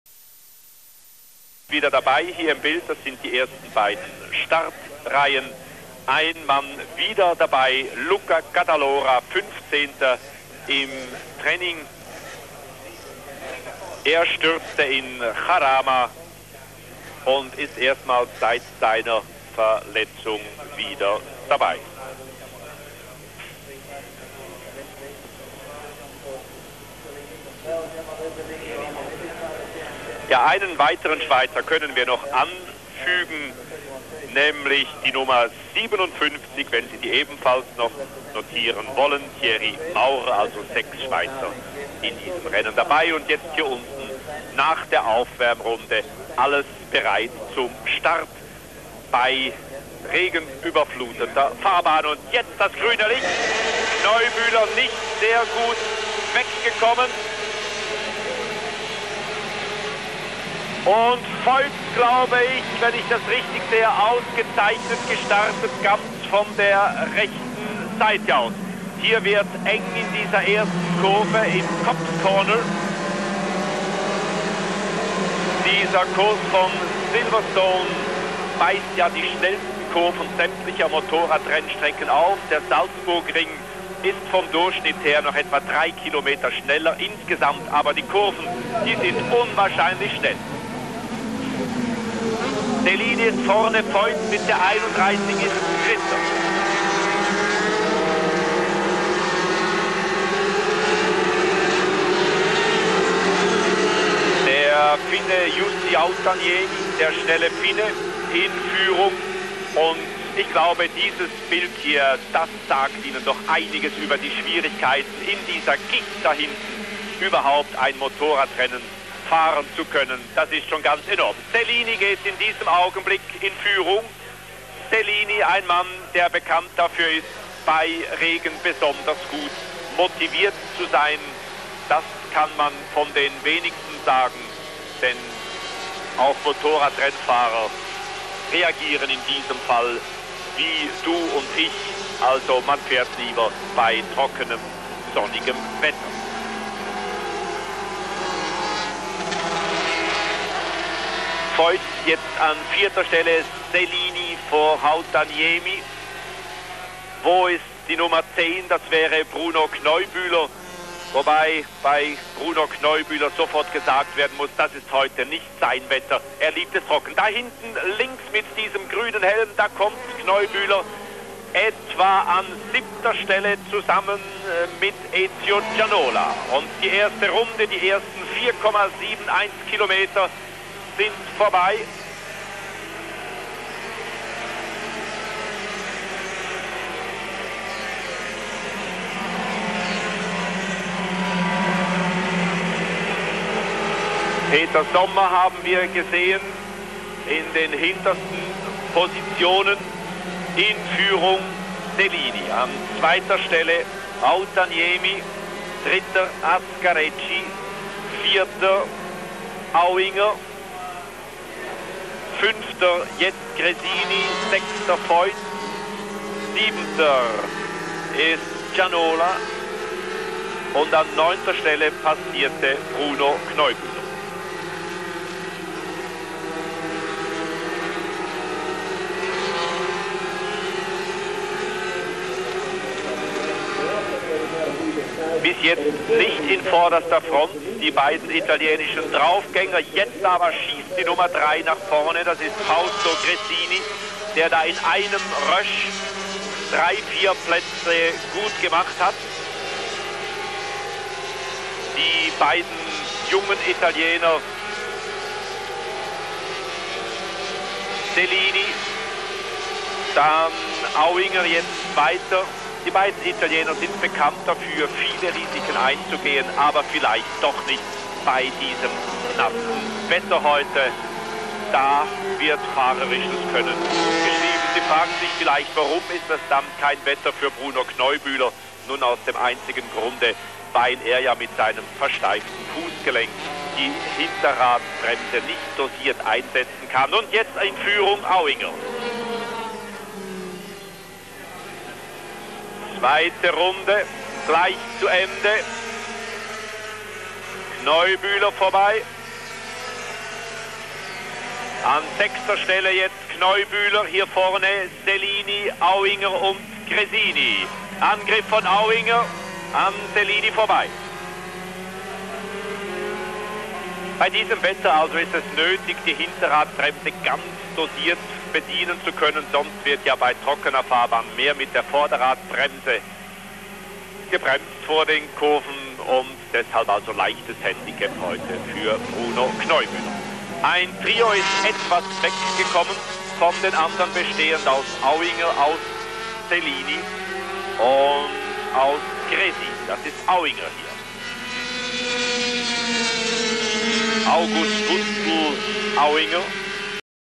RR WM d ´85 Siverstone 125cc - Auinger Sieg (Start)
Motorrad Weltmeisterschaft 1985, Silverstone, 125ccm, 4aug85, 1. Auinger, 2. Bianchi, 3. Selini, 4. Gresini, 5. Hautaniemi, 6. Brigaglia, 7. Perez, 8. Liégeois, 9. Hupperich, 10. McGarrity